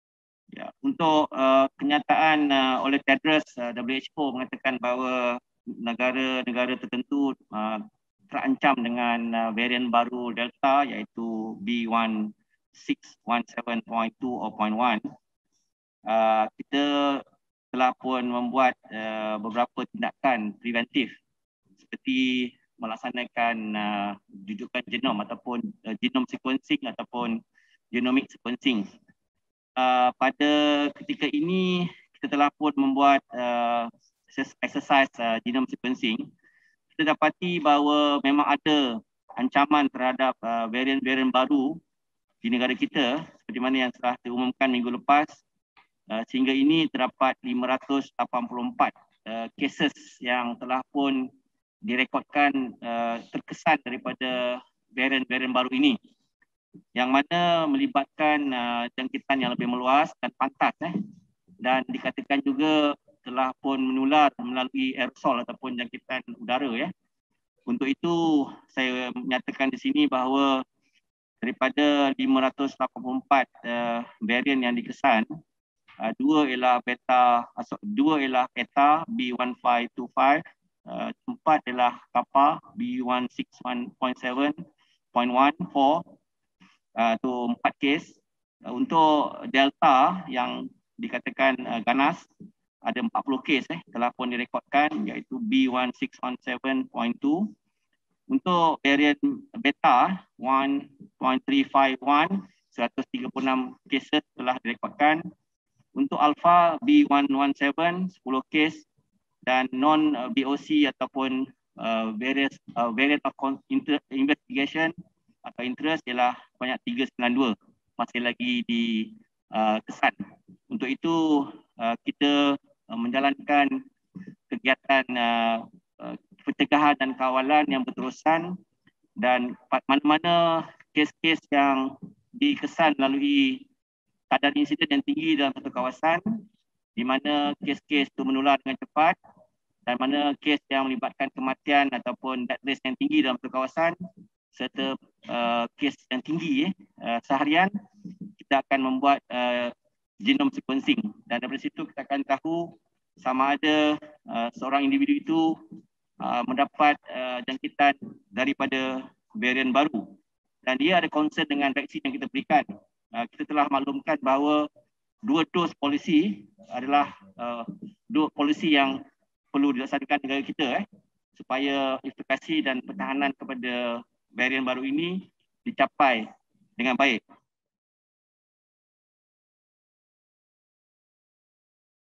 Covid-19 variants detected in 584 cases so far, says Health Minister
Covid-19 variants have been detected 584 cases in Malaysia so far, says Health Minister Datuk Seri Dr Adham Baba.